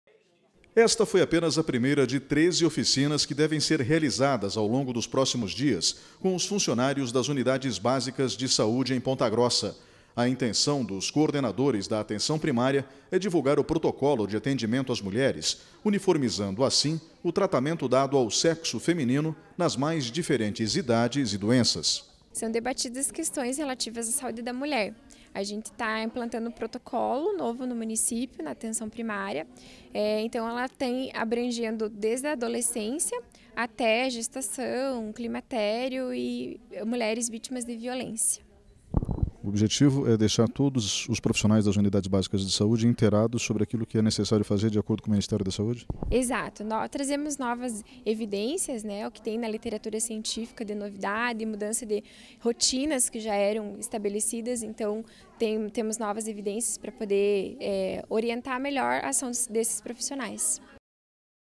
OFICINA SAÚDE MULHER
NESTA QUARTA-FEIRA A SECRETARIA MUNICIPAL DE SAÚDE COMEÇOU A REALIZAR OFICINAS PARA MÉDICOS E ENFERMEIRAS DAS UNIDADES BÁSICAS.

O TRABALHO VISA REPASSAR INFORMAÇÕES ATUALIZADAS SOBRE A SAÚDE DA MULHER.